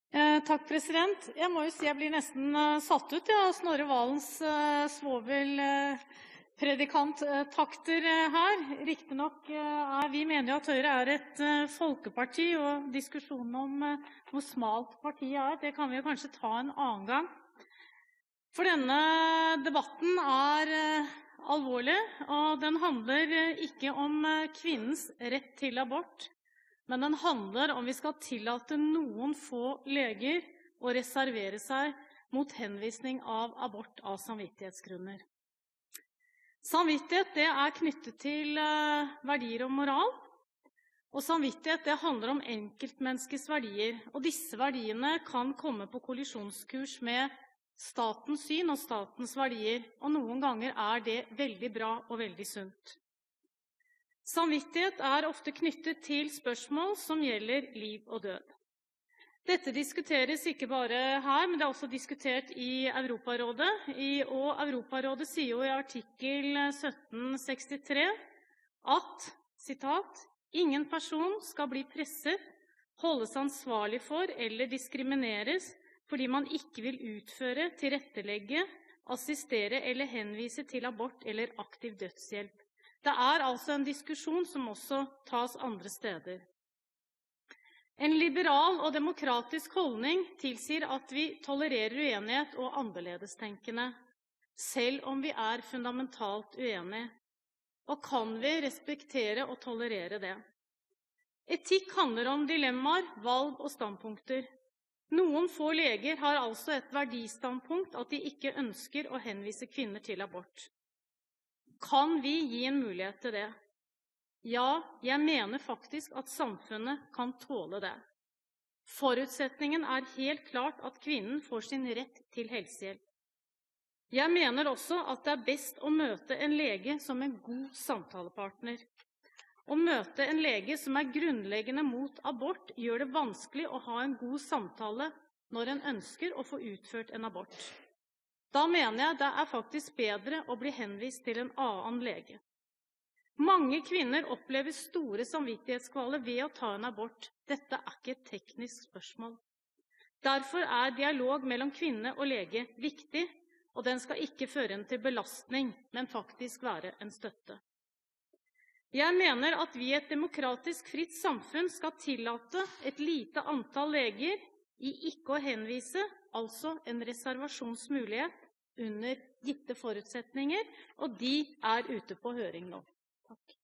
27.02.2014 Sak 1 7 Kristin Ørmen Johnsen
Om å ikke utvide legers reservasjonsrett når det gjelder henvisning til abort, utskriving av resept på p-piller eller henvisning til assistert befruktning